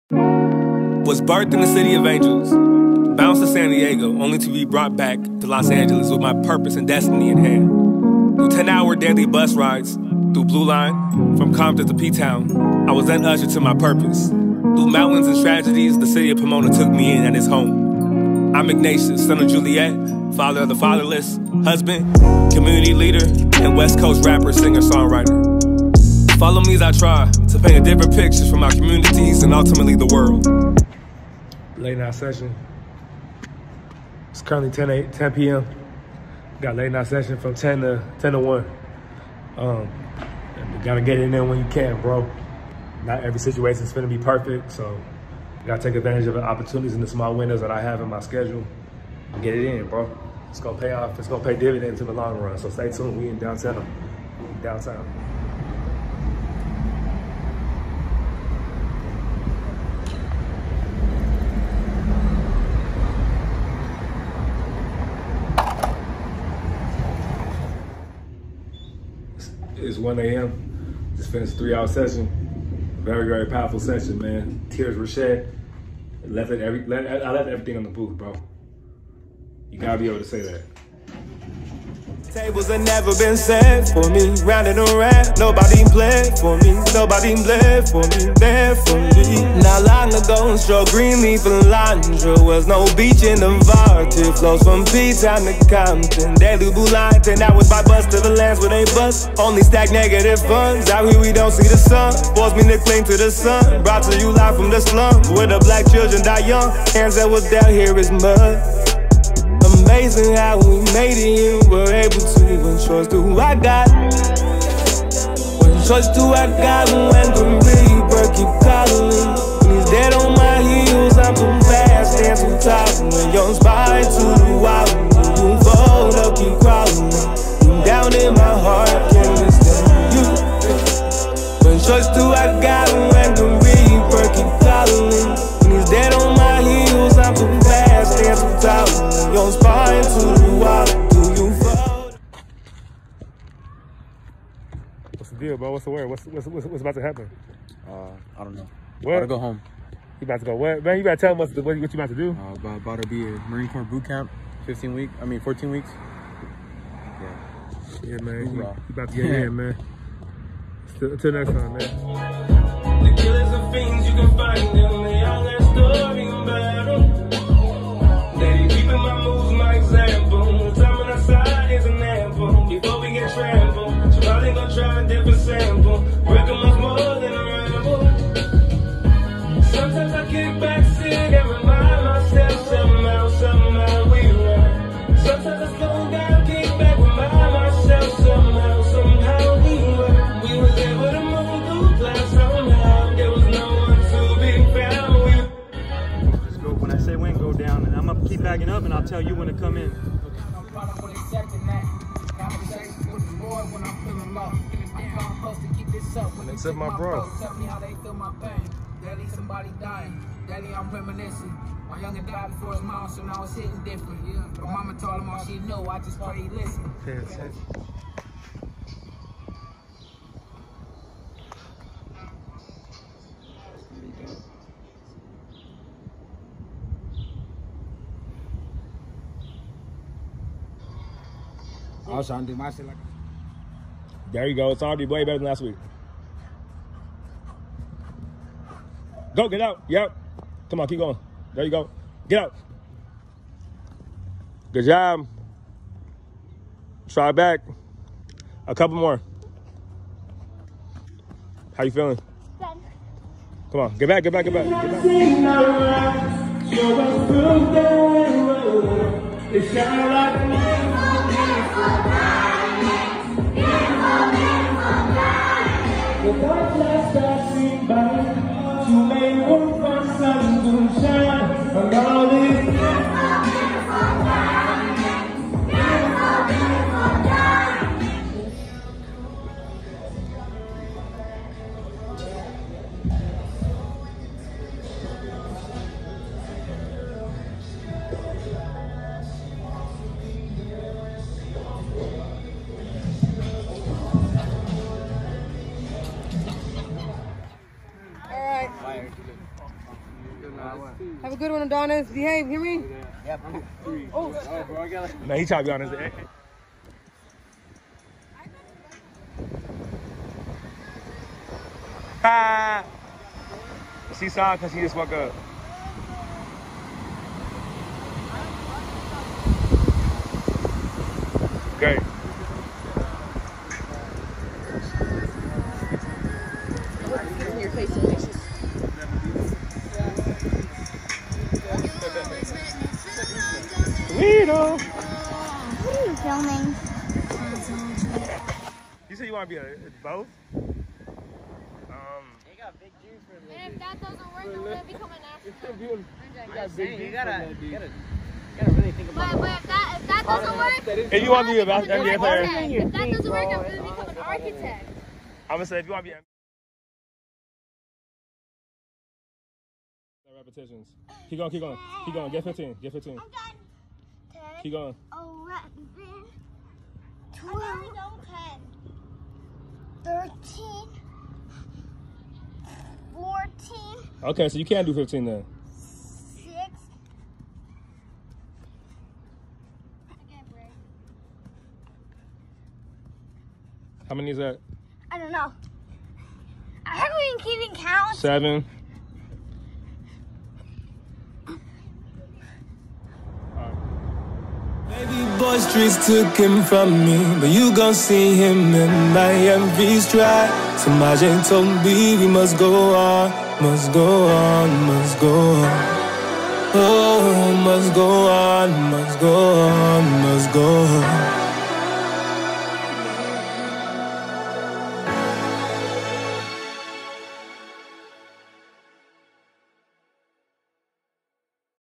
Vlognatius 8
Ignatius records new music and spends time with loved ones.